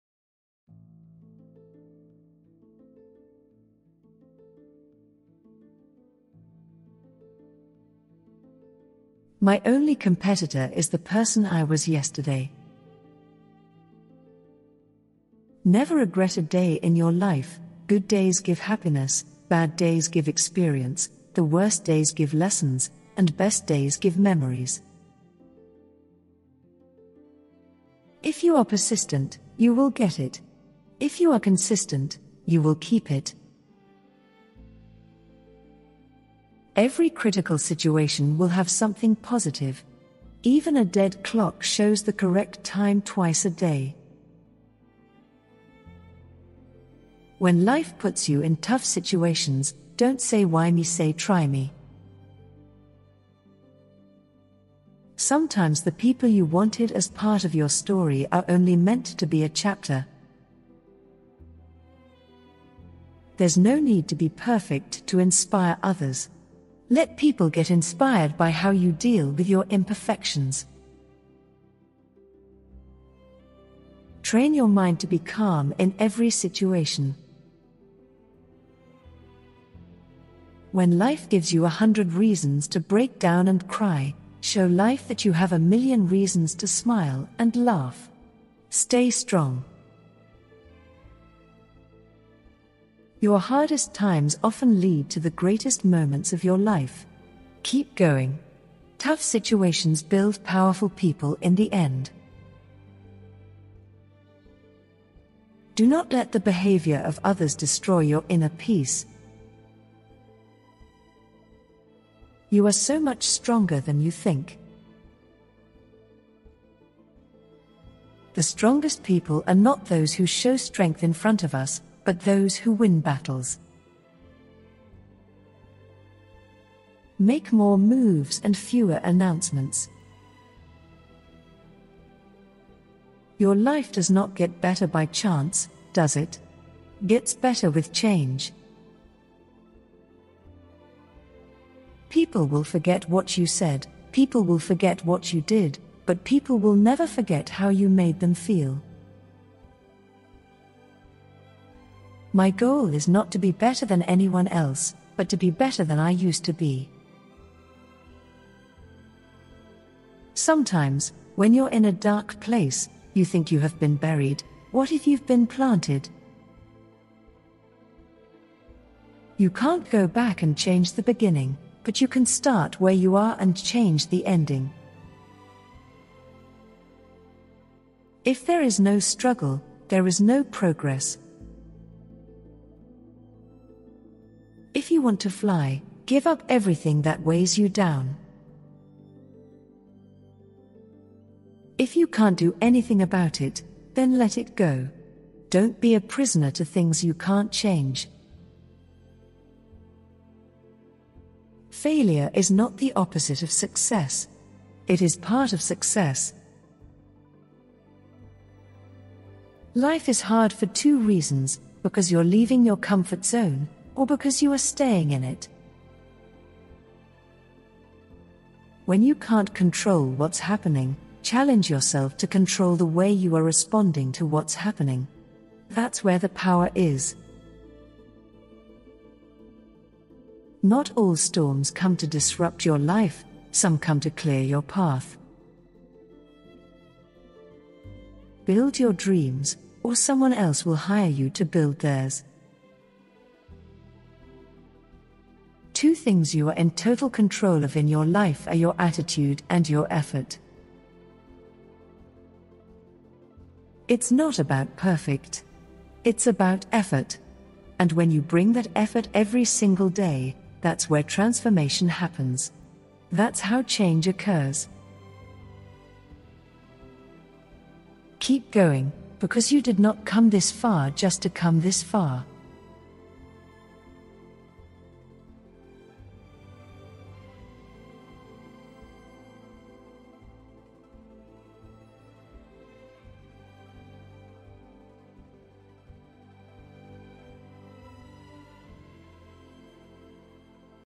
Motivational Quotes for Life: Powerful Quotes to Give You Strength When You Need It Most
Life has a funny way of testing us. We are constantly presented with obstacles that we must somehow find the strength to overcome.